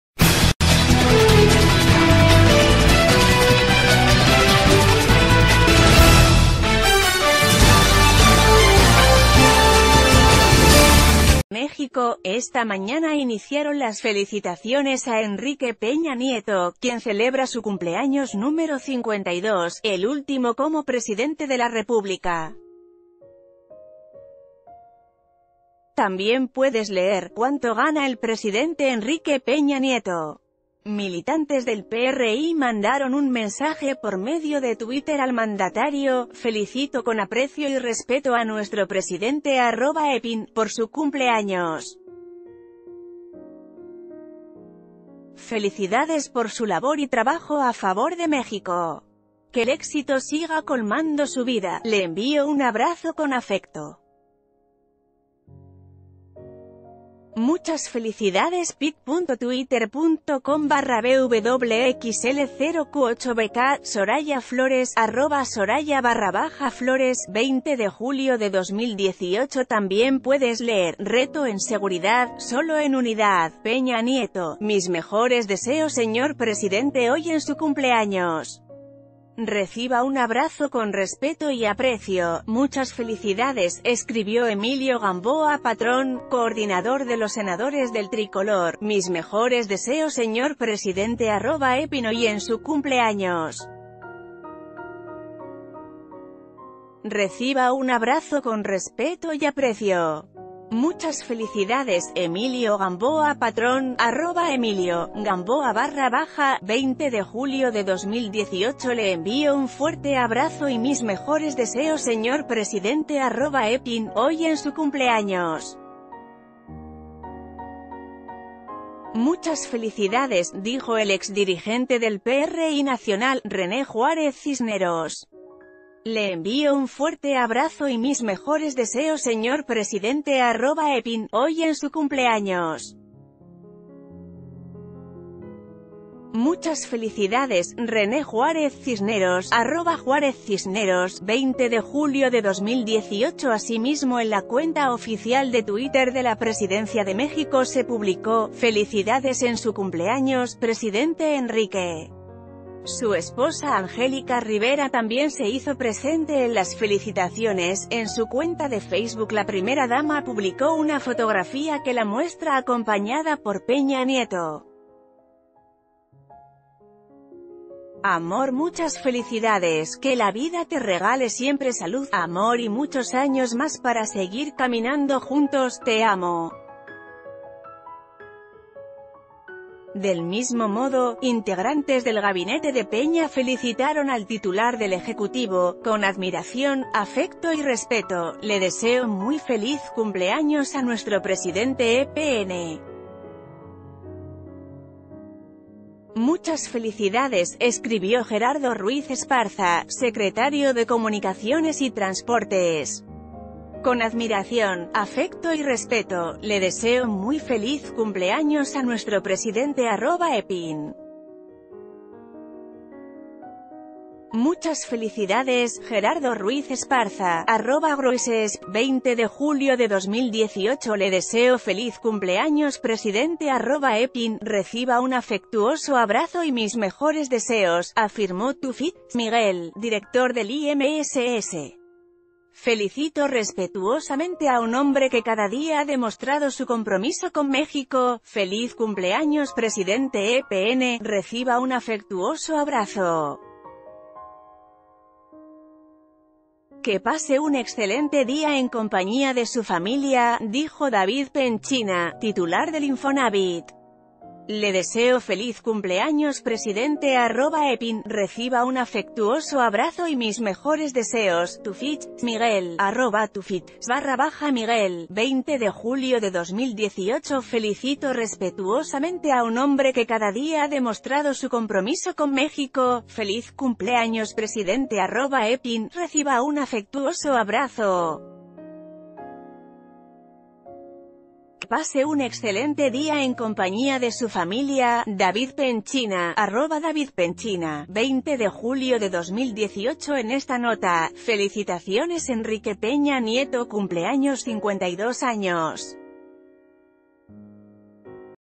Enrique Peña Nieto celebra su cumpleaños número 52
México.- Esta mañana iniciaron las felicitaciones a Enrique Peña Nieto, quien celebra su cumpleaños número 52, el último como Presidente de la República. También puedes leer: ¿Cuánto gana el presidente Enrique Peña Nieto. Militantes del PRI mandaron un mensaje por medio de Twitter al Mandatario. Fel...